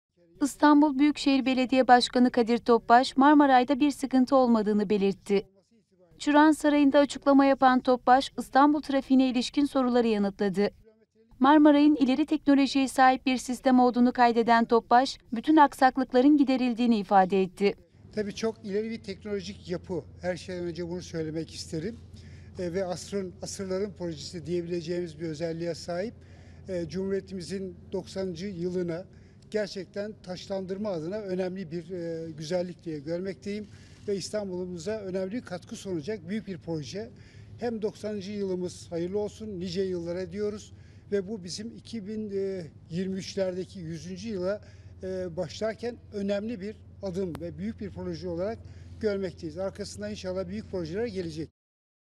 Topbaş, 'Marmaray, dünyanın ilgisini çeken önemli bir ulaşım sistemidir'
İstanbul Büyükşehir Belediye Başkanı Topbaş, Marmaray'da bir sıkıntı olmadığını belirterek,

marmaray,kadir topbaş,asrın projesivideosu,video,haber,haberi,videolu haber